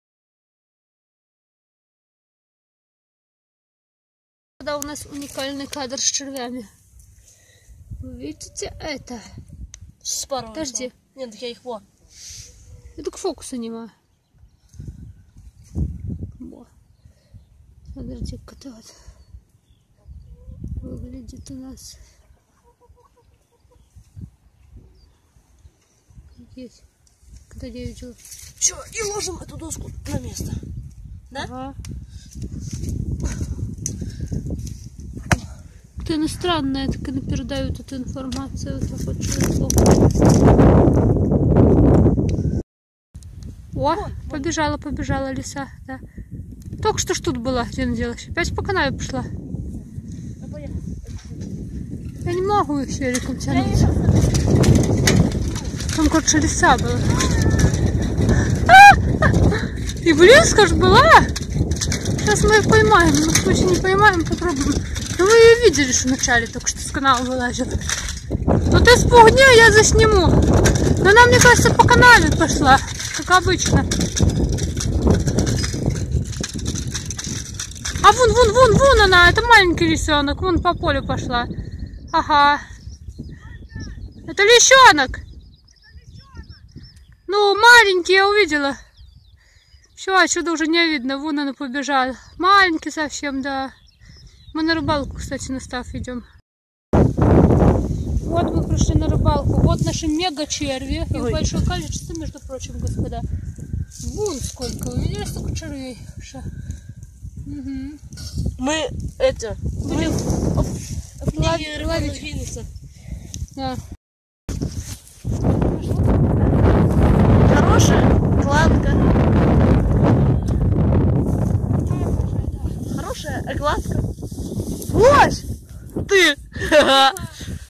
как спариваются черви
Вы когда-нибудь видели, как спариваются черви? А лису в поле? Посмотрите тогда.